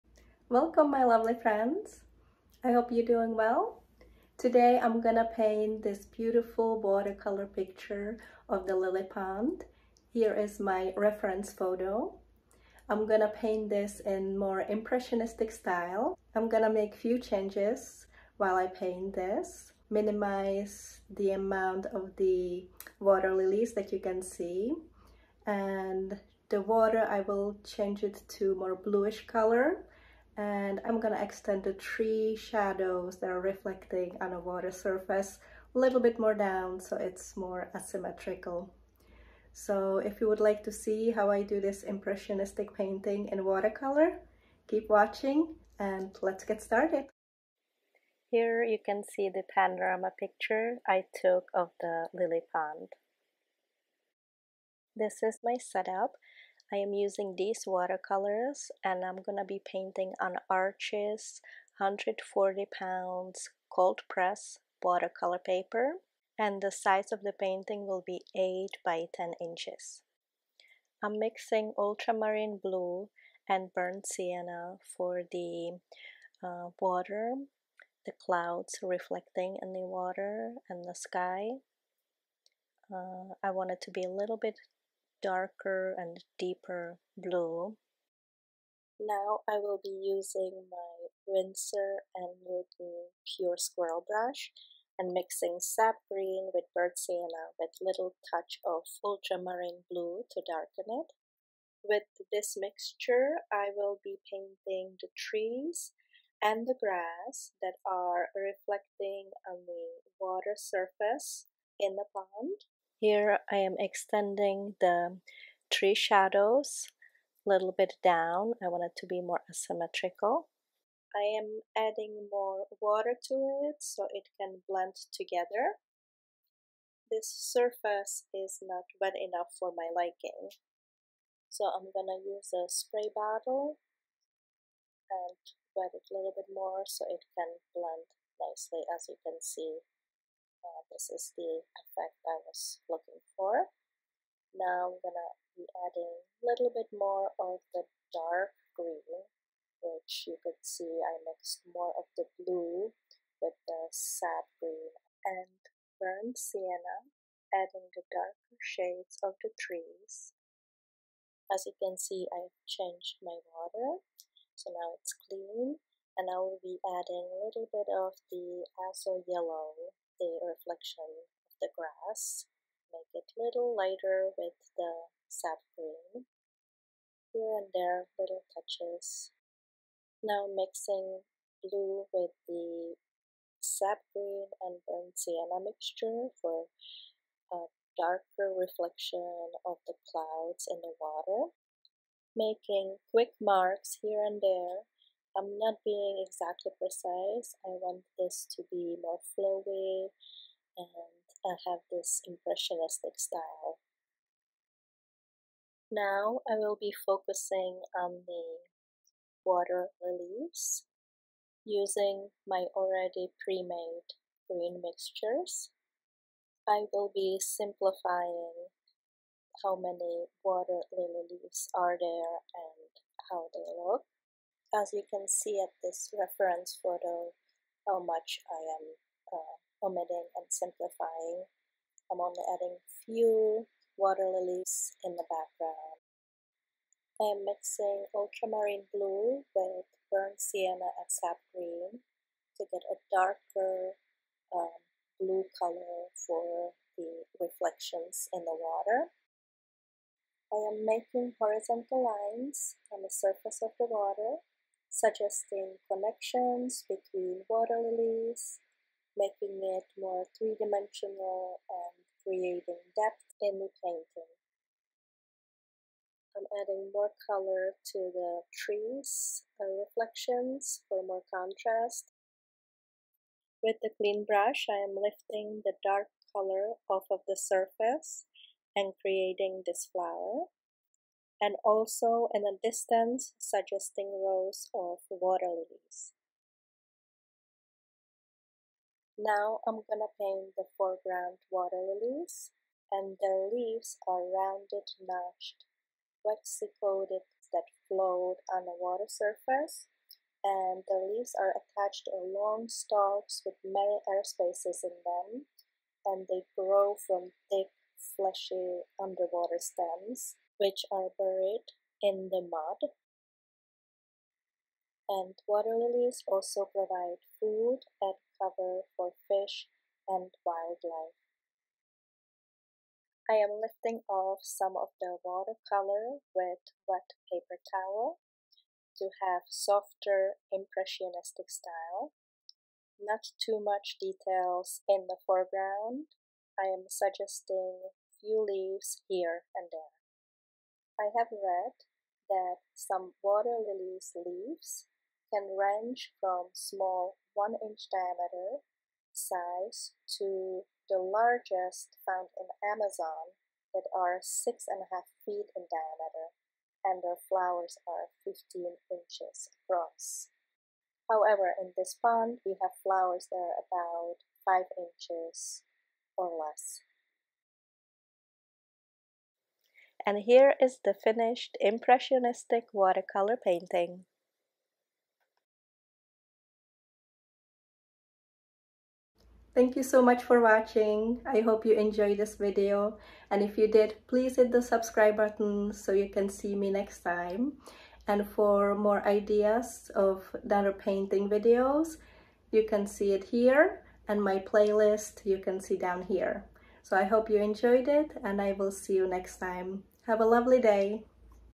IMPRESSIONISTIC WATERCOLOR - WATER LILY POND - Tutorial step-by-step painting
I'm painting impressionistic style watercolor of a water-lily pond. I took my refference photo in Chicago Botanical Garden.

  In this video I'm using:
M. Graham watercolors
Arches 140lb Cold Press Watercolor Paper
Winsor & Newton Pure Squirrel Brush #250
Wash Brush size 3 inches